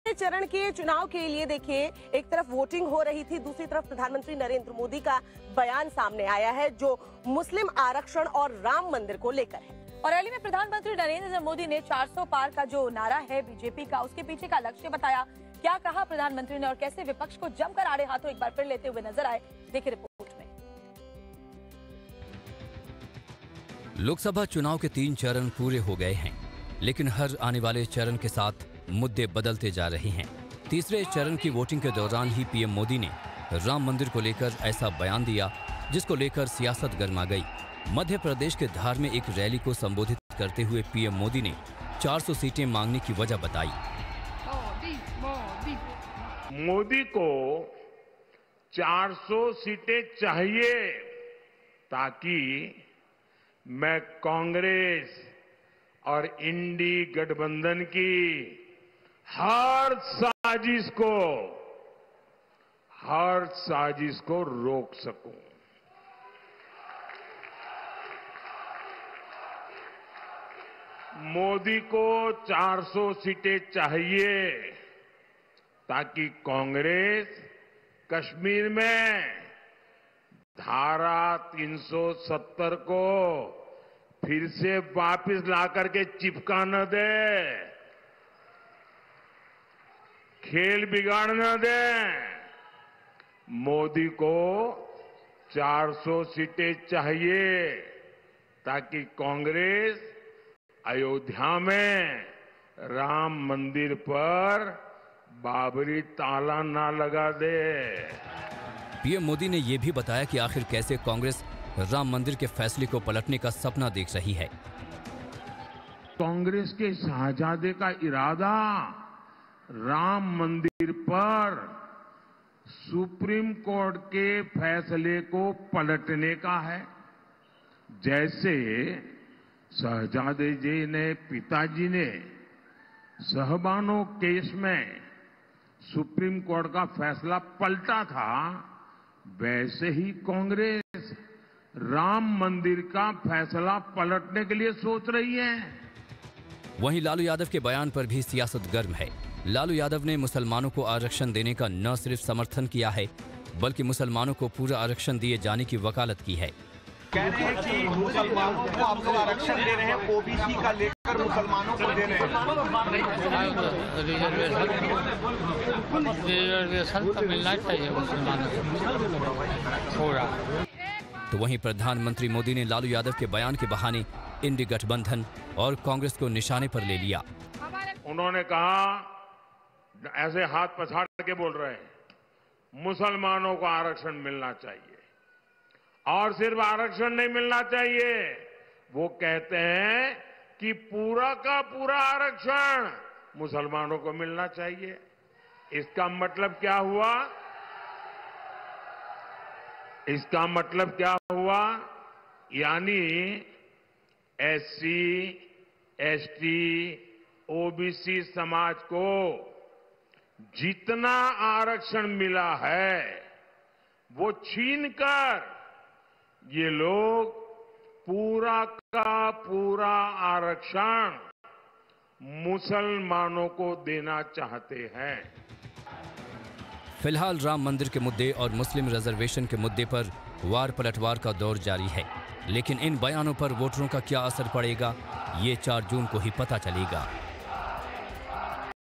PM Modi का विपक्ष पर जोरदार हमला, कहा इस बार का चुनाव संतुष्टिकरण और तुष्टिकरण के बीच | Hindi News
PM Modi Attack On Opposition News | Maharashtra के Beed के चुनावी रैली से PM Modi का विपक्ष पर जोरदार हमला, पीएम ने कहा कि इस बार का चुनाव संतुष्टिकरण और तुष्टिकरण के बीच है, Ram Mandir पर Babari ताला ना लगे इसलिए चाहिए 400 से ज्यादा सीटें, Congress बोलीं देश को गुमराह कर रहे है पीएम, देखिए पूरी ख़बर...